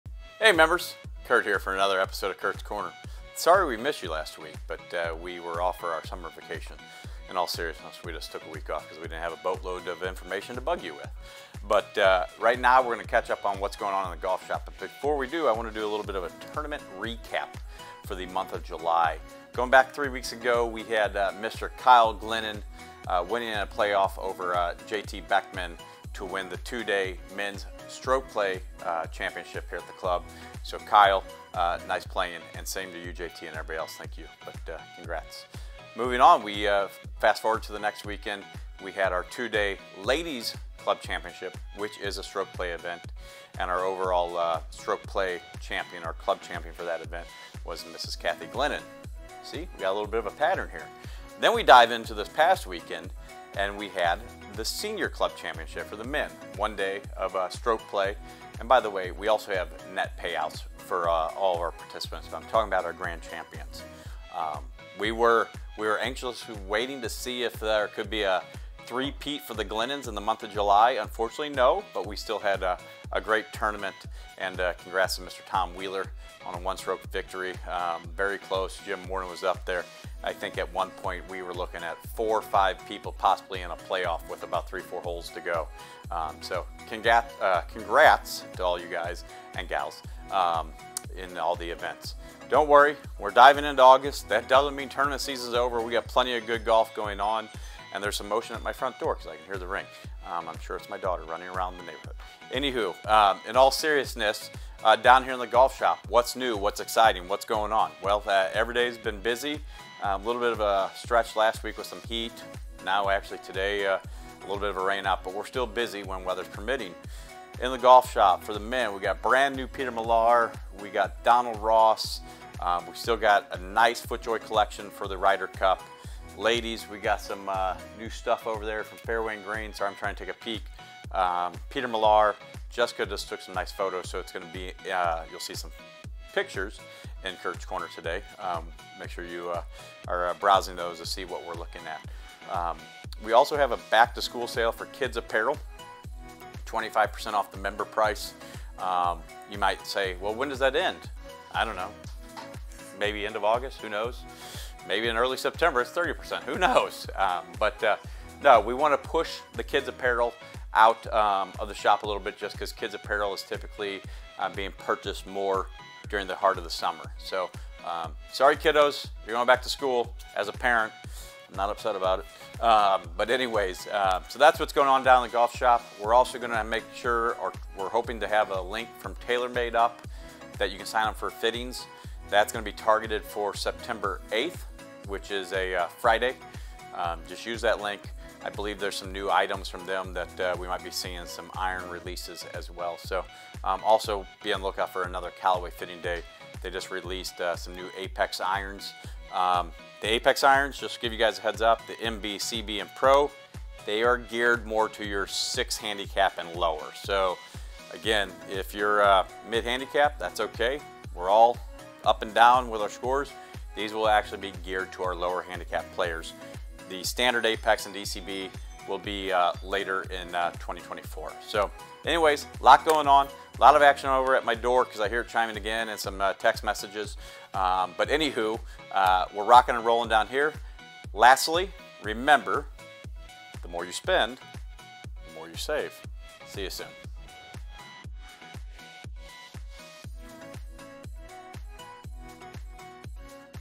Curt's Corner
Curt congratulates our golf champions! Look for the link to our TaylorMade Fitting Day in September.